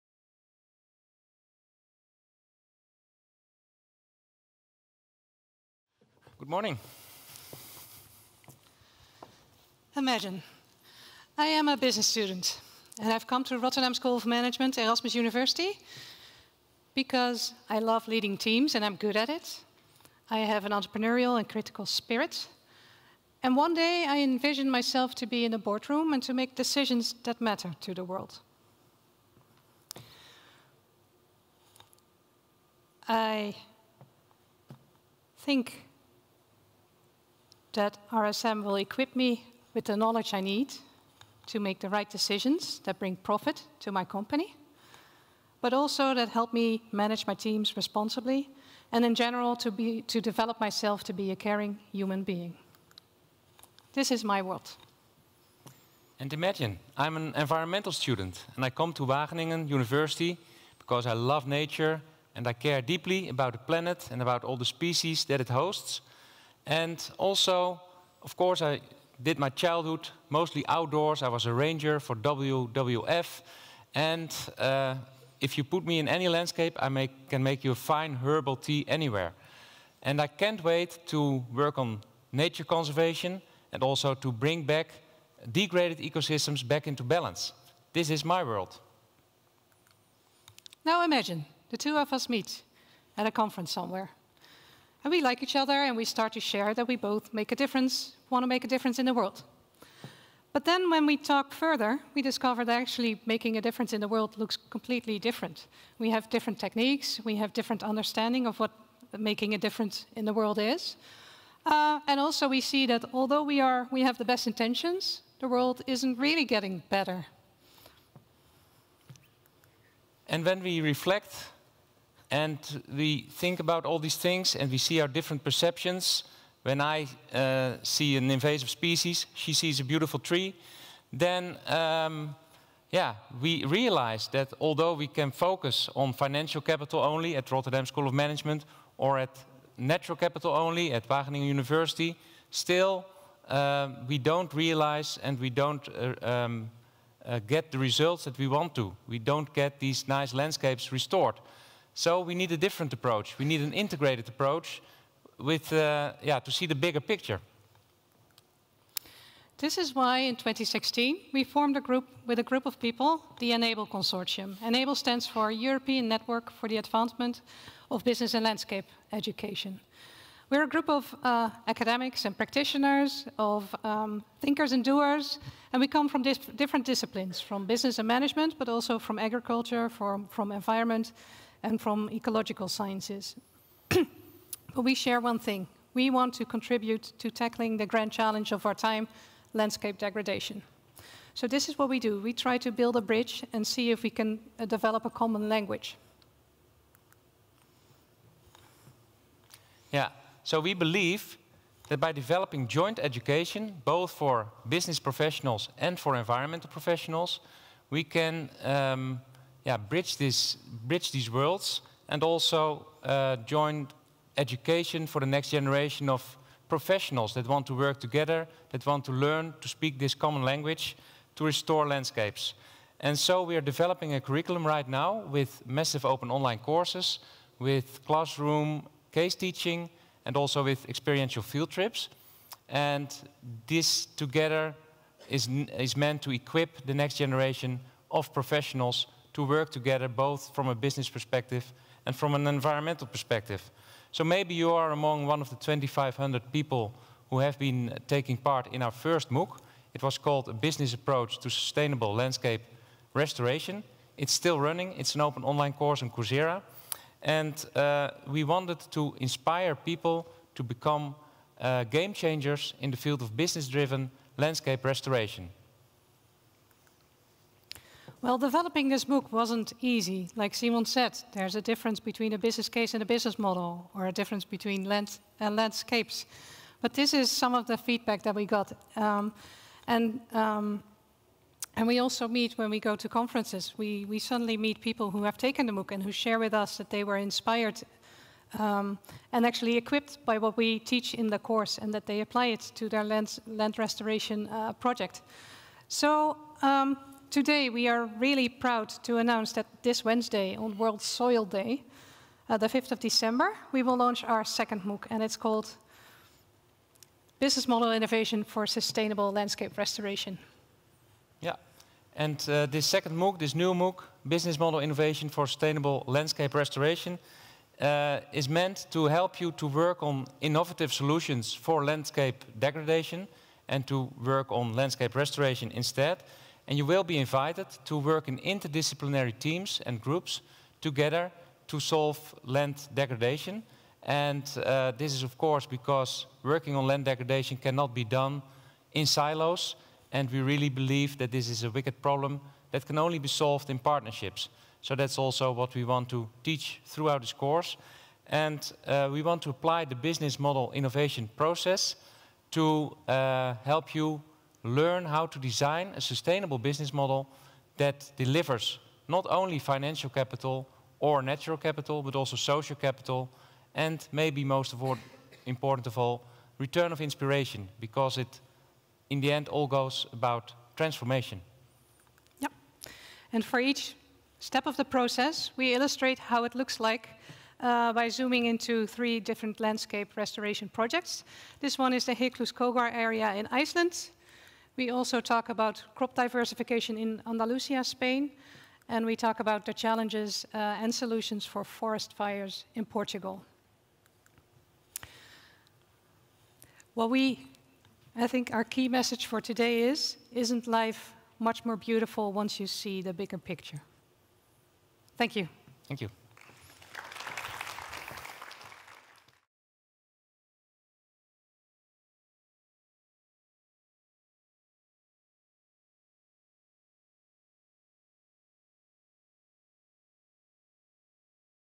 Eva Rood and Simon Moolenaar - The joys of developing transdisciplinary landscape education
Various solutions to land restoration exist already. What is needed is upscaling through education, partnerships and sustainable business models to move from pledges to implementation to tackle the grand challenge of land restoration globally. The private sector has financial resources for scaling up land restoration. What is vital is that business professionals are educated about the importance of healthy ecosystems / sustainable business models; and that environmental professionals are aware of the valuable contribution businesses and business model thinking can make. Through its MOOC on Business Model Innovation for Land Restoration, ENABLE, the European network for the advancement of business & landscape education, aims to inspire thousands of business and environmental students and professionals who are the leaders of today and tomorrow driving land restoration and sustainable land management. The ENABLE project is able to foster learning and knowledge sharing through its public, private and non-profit partners providing various resources and knowledge, and its mission of providing high quality open access education platforms. This is done by developing various education tools that are educating students and professionals about 4 returns thinking: a business model can only be healthy if it delivers 4 returns – return of financial capital, return of natural capital, return of social capital and return of inspiration.

Speaker: Eva Rood, Project Leader, Rotterdam School of Management, Erasmus University, Commonland, ENABLE